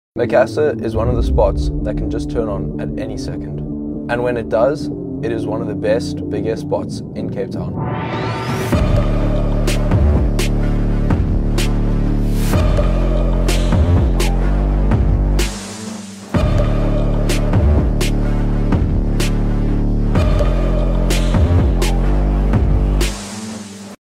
Ride Waves with Jason: Kitesurfing Cape Town Ep.2
🏄‍♂️Get ready to ride the waves with Jason Vanderspuy in the exciting second episode of Maps of Home🌊. Feel the thrill of kitesurfing in the beautiful location of Cape Town, Western Cape. 🌍

🎬 This project has been a labor of love for Luca and me, working together with the talented @nomadicsea this season in the breathtaking vistas of Cape Town.

Come and join us on an epic journey that takes us to our four most-loved kitesurfing spots! 📍 The whole series is an adrenaline rush that you absolutely should not miss out on!

Seize the wind and let's ride together! 🪁✌️

📸 Pictures superbly captured by @nomadicsea
and @mitchelldoylemarkgraaff.